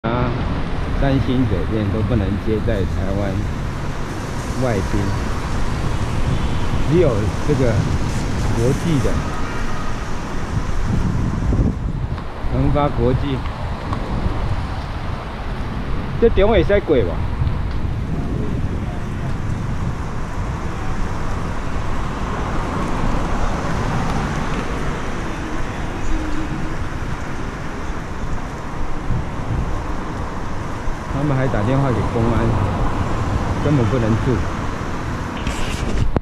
絲路三劍客--甘肅省隴西縣--恆發國際大酒店 20180909
隴西連速8都不接待外賓，既然已經通報公安，為避免麻煩只好入住這次行程比較貴的酒店，記得是接近300人民幣左右，一般不超過100